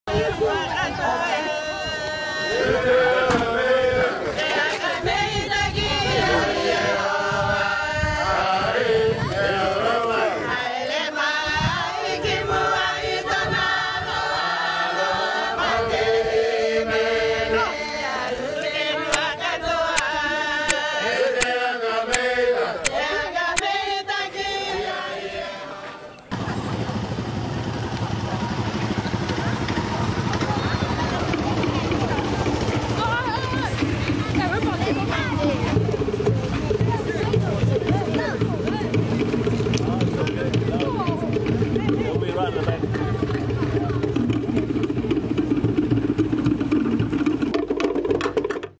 Jerome Kaino and the RWC at Takamoa, Rarotonga
-All Black Jerome Kaino brought the Rugby World Cup to Rarotonga to show it to the All Blacks' biggest fans - Cook Islanders. This video is the start of the parade through Avarua township.